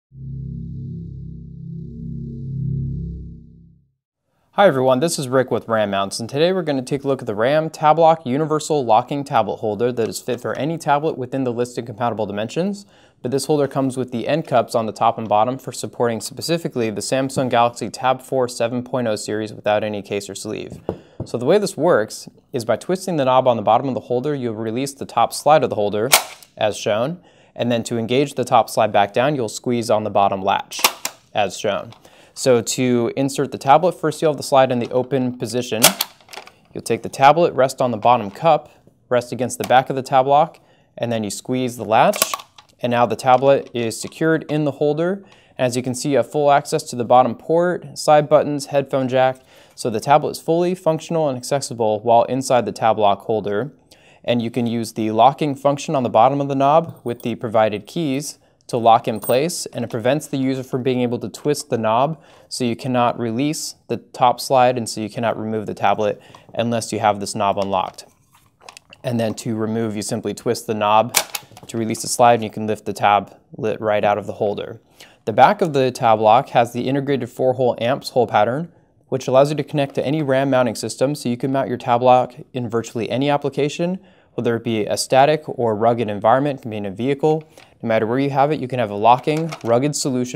RAM Tab Lock™ Locking Cradle for 7 Tablets including the Samsung Galaxy Tab 4 7 0
Go ahead and watch this short video to learn all about the RAM tab lock. This video will describe to you information on how to use the product and what it does for you Samsung Galaxy Tablet.